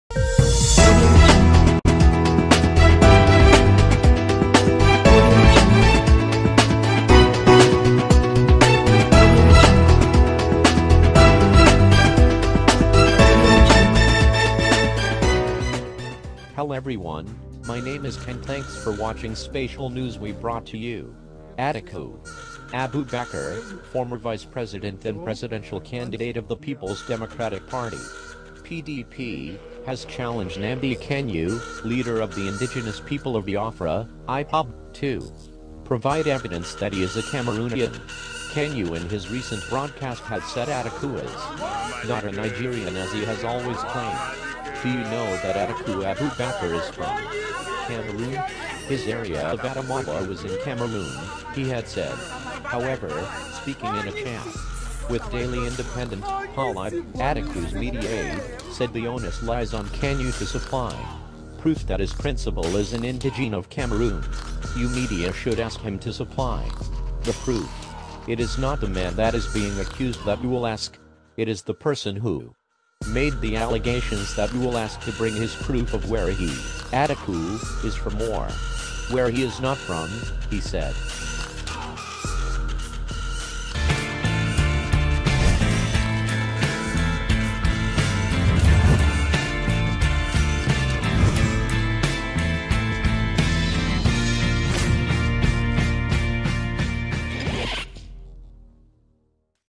Atiku Replies IPOB Leader Nnamdi Kanu: I Am Not From Cameroon - Fired Back
Atiku Abubakar, former Vice-president and presidential candidate of the Peoples Democratic Party (PDP) has challenged Nnamdi Kanu, leader of the Indigenous People of Biafra (IPOB) to provide evidence that he is a Cameroonian.
Kanu in his recent broadcast had said Atiku is not a Nigerian as he has always claimed.
“Do you know that Atiku Abubakar is from Cameroon? His area of Adamawa was in Cameroon,” he had said.
However, speaking in a chat with Daily Independent, Paul Ibe, Atiku’s media aide, said the onus lies on Kanu to supply proof that his principal is an indigene of Cameroon.
“You media should ask him to supply the proof. It is not the man that is being accused that you will ask. It is the person who made the allegations that you will ask to bring his proof of where he (Atiku) is from or where he is not from,” he said.